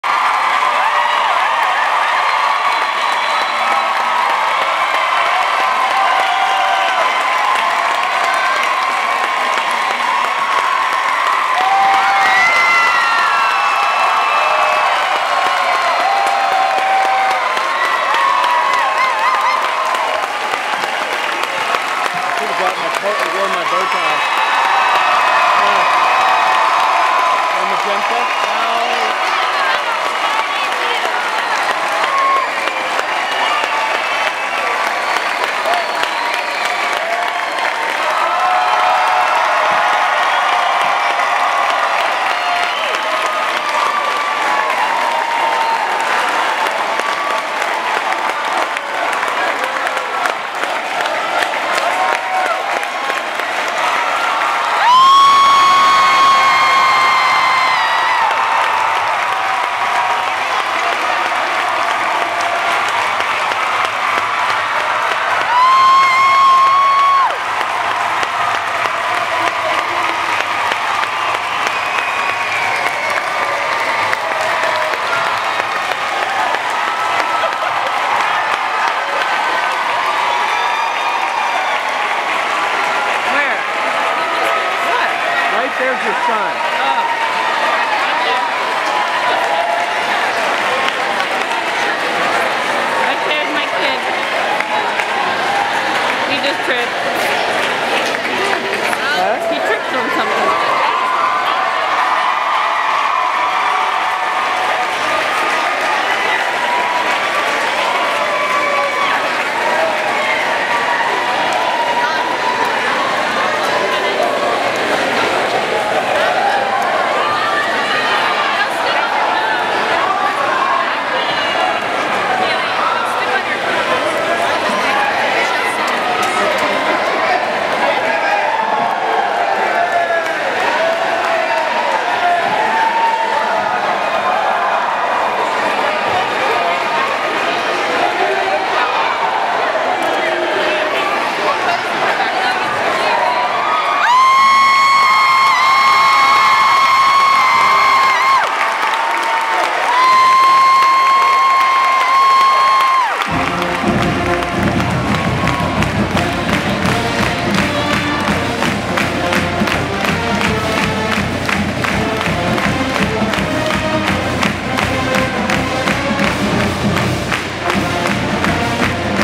Wakeland HS pep rally.
Pink out for the girl's basketball coach who has cancer and channel 8 news was there as well.  Noah came out with the cross country team.